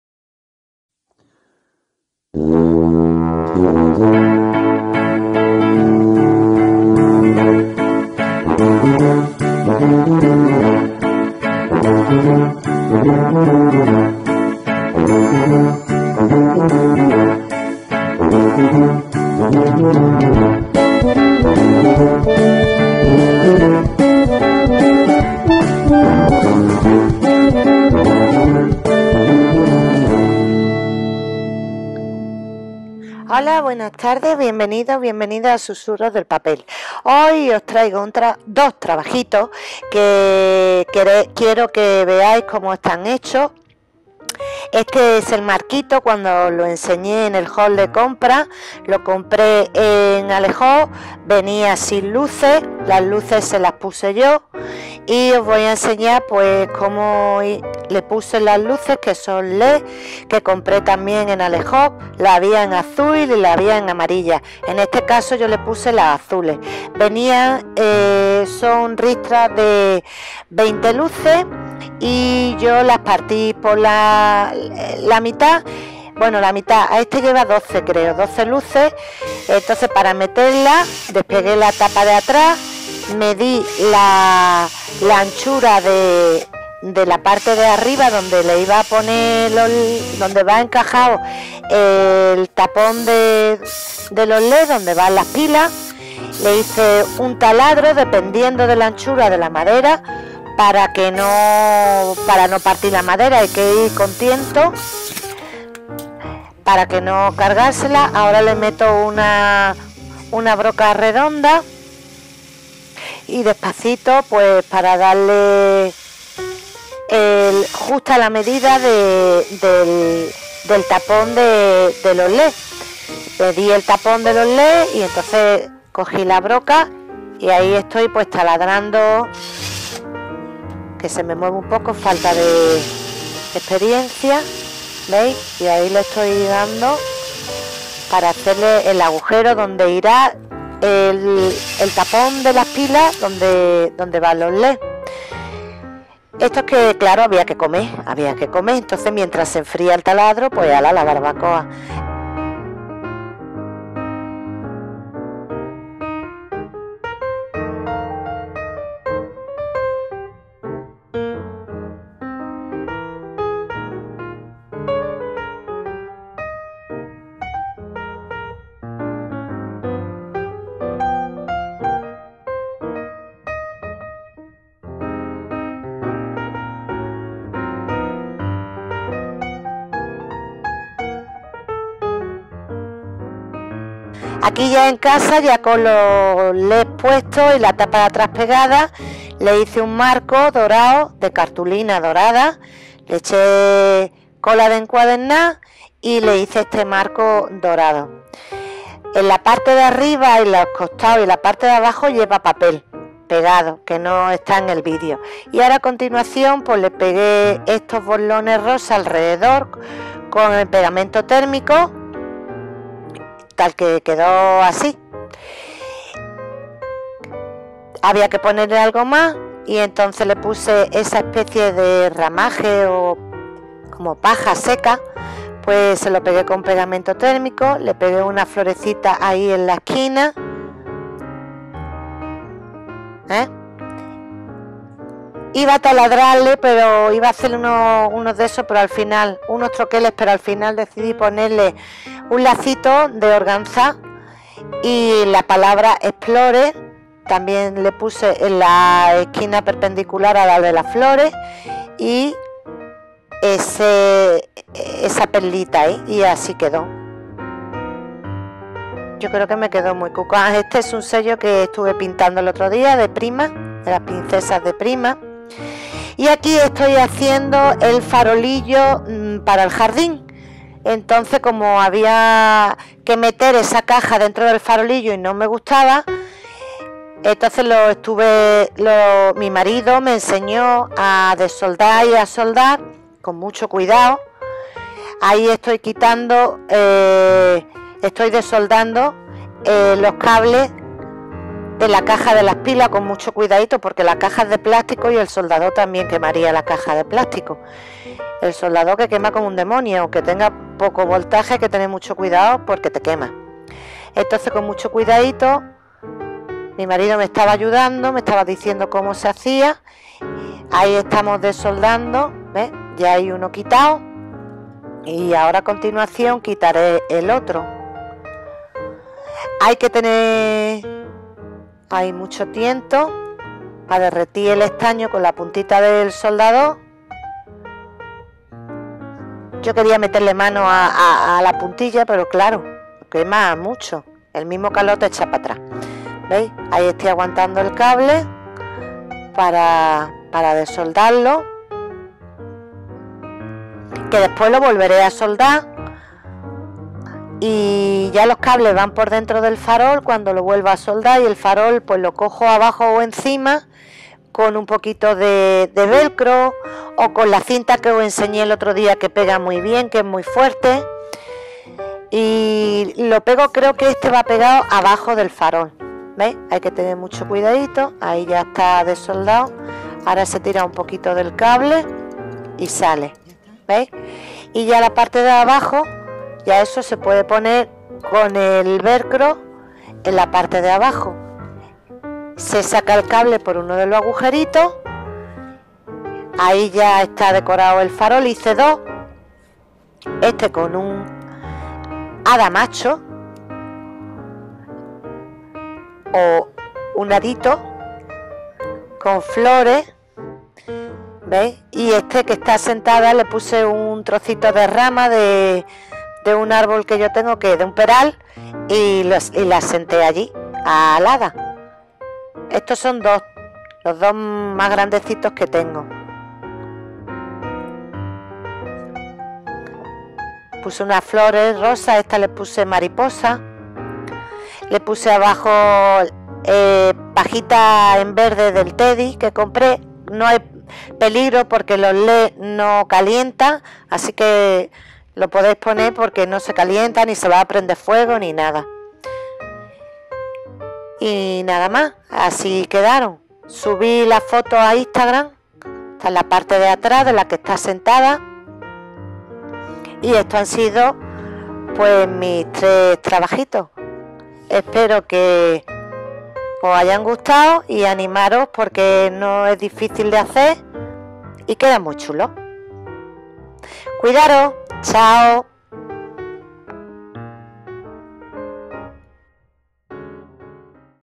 SuSuRRoS DeL PaPel-Scrap ESPAÑA- Haz tus objetos de decoración con herramientas de Lidl
Esta vez Son marcos y faroles de jardín alterados por un@mismo. Podréis crear vestro propios diseños y adornar cualquier rinconcito